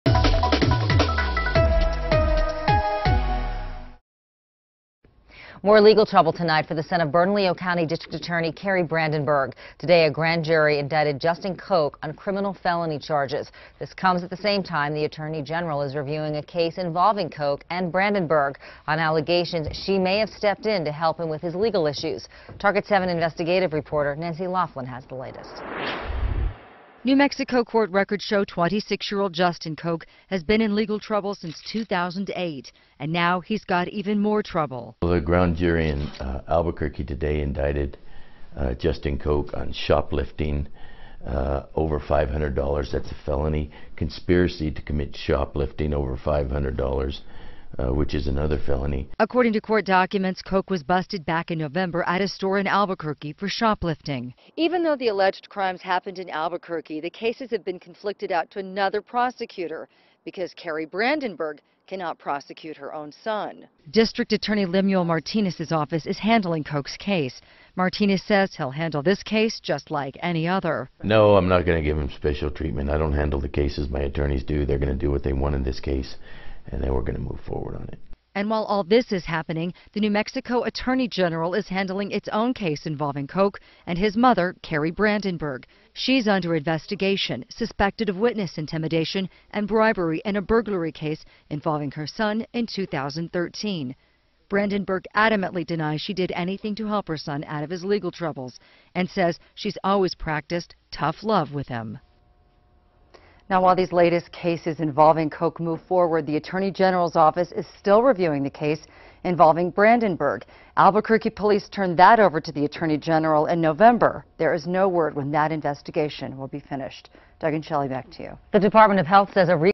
DA's son indicted on criminal felony charges
The son of Bernalillo County's district attorney was indicted on criminal felony charges Tuesday.